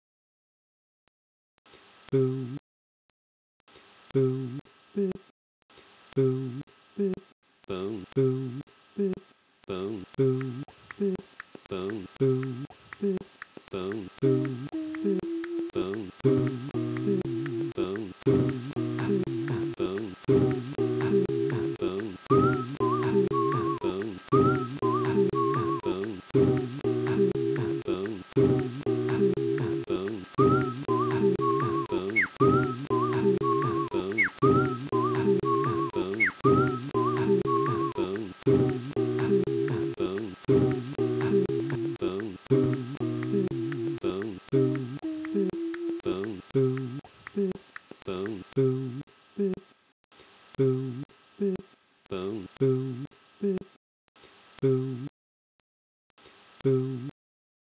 Whistle-happy with MmmTsss
A little improvisation I did using my MmmTsss software.